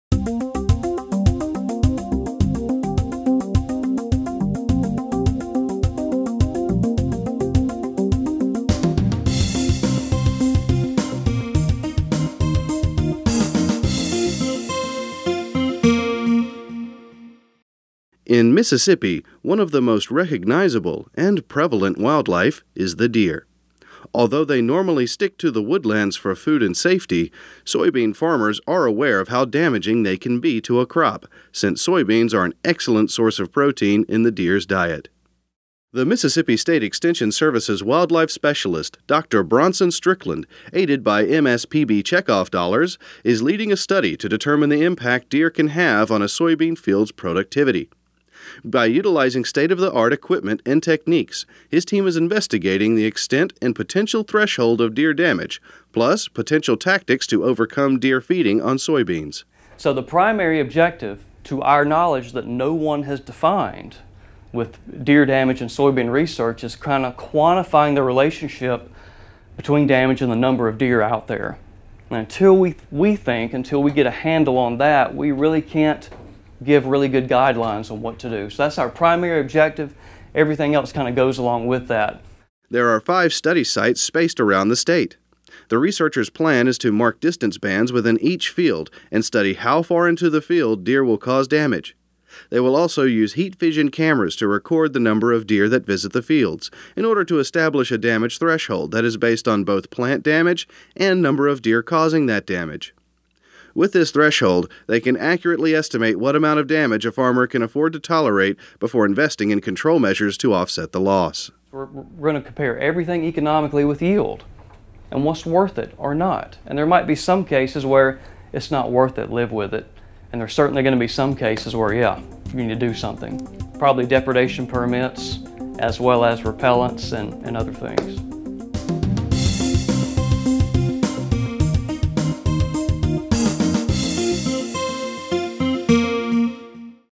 Project Overview: Estimation of Deer Damage to Soybean Production in Mississippi
The white-tailed deer is one of Mississippi's most valuable wildlife resources.   However, to the states' soybean farmers, deer are often considered a pest since they consume and thus damage soybeans in fields that are in close proximity to their habitat. To quantify this damage and its economic impact, the MSPB funded a project to estimate the amount of deer predation in soybean fields and measure the potential negative impact of this feeding on soybean yield. The ultimate goal of this study is to relate indices of deer abundance to soybean depredation so that a consumption rate can be determined. This consumption rate can then be used to measure the potential response to methods that may be used to prevent or mitigate deer feeding damage to the soybean crop.